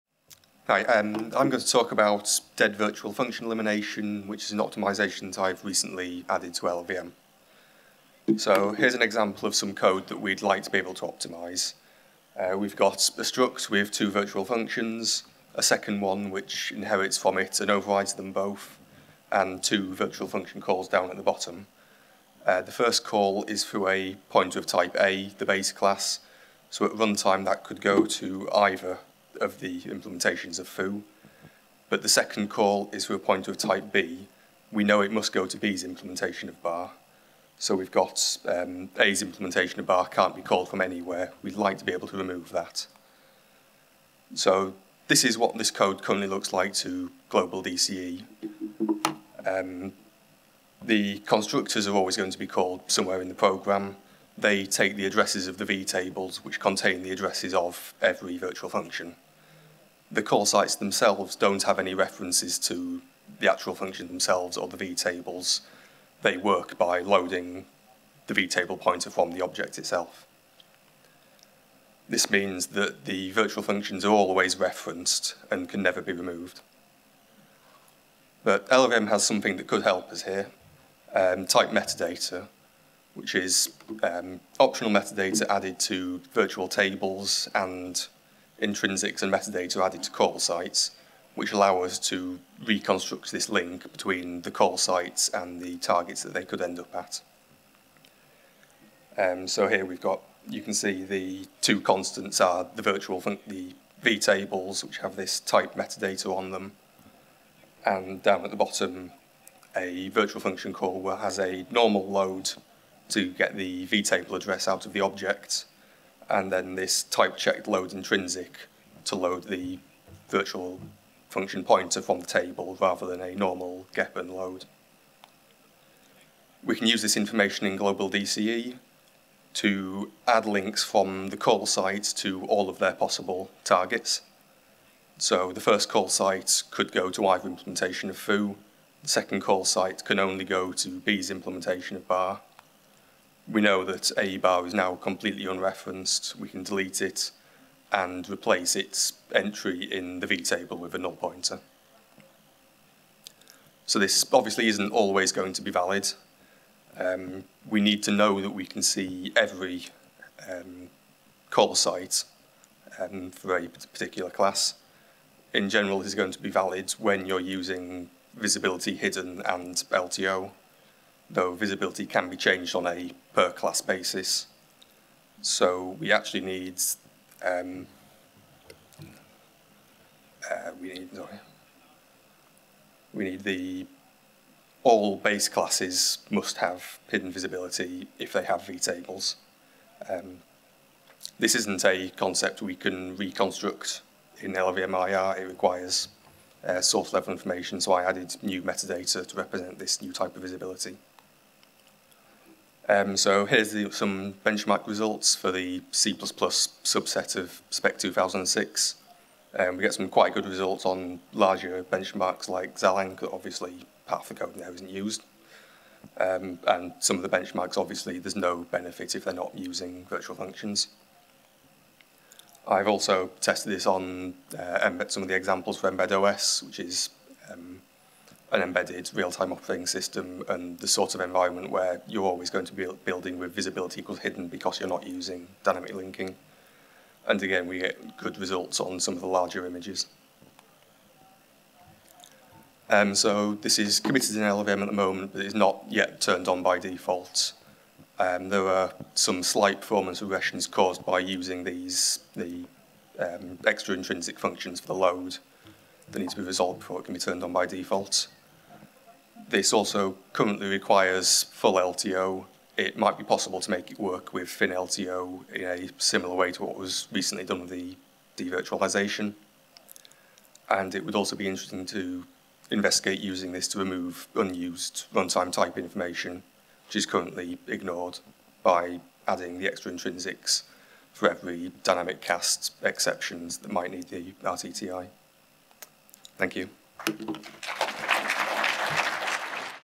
2019 LLVM Developers’ Meeting: O. Stannard “Virtual Function Elimination in LLVM”
Virtual Function Elimination in LLVM  - Oliver Stannard

I am currently working on a patch to LLVM and Clang to allow unused C++ virtual functions to be removed by LTO. I will explain what virtual functions and calls to them look like in LLVM IR, and why they can't be removed by existing optimisations. Then I will describe the changes I have made to LLVM and Clang to make this optimisation possible.